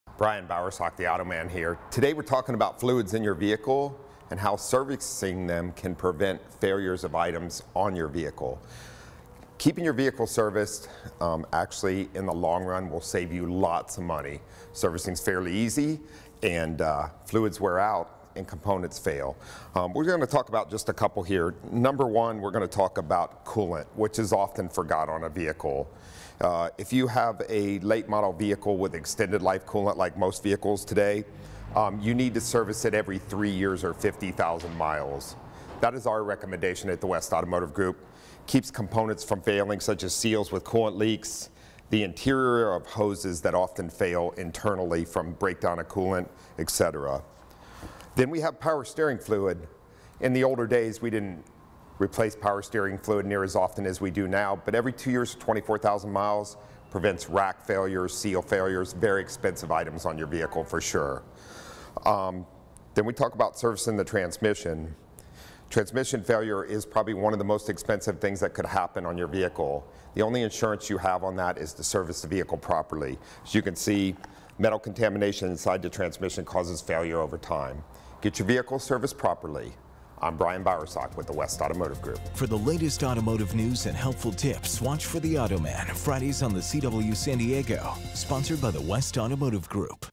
Properly servicing your vehicle's fluids
Tune in and turn on the Auto man this Friday Morning or Evening as we are discussing: Properly servicing your vehicle's fluids. Tune in this Friday morning and catch us on the San Diego CW.